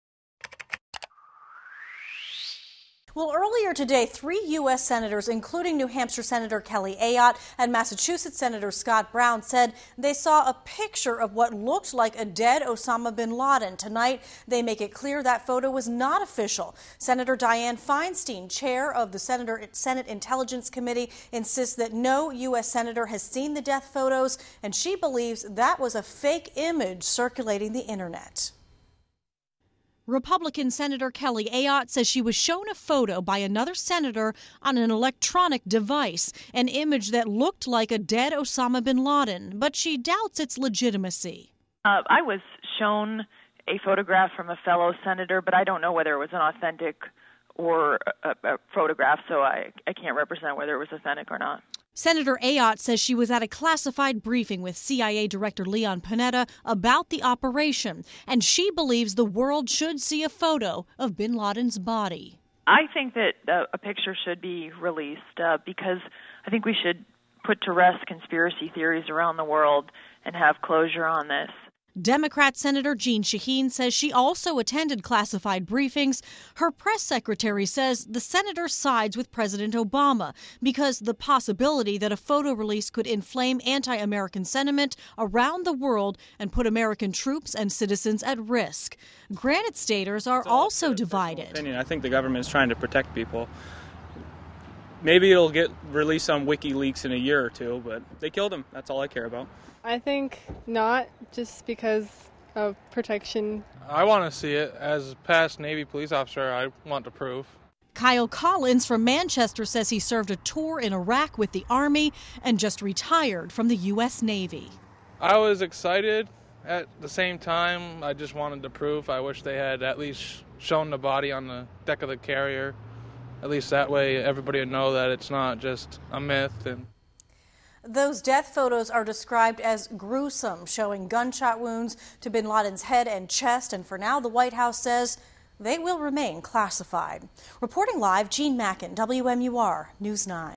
Public Servants React To Death-Photo Debate
A debate is going on across the country about whether Osama bin Laden's death photos should be released to the public to quell doubts that the al-Qaida leader is still alive.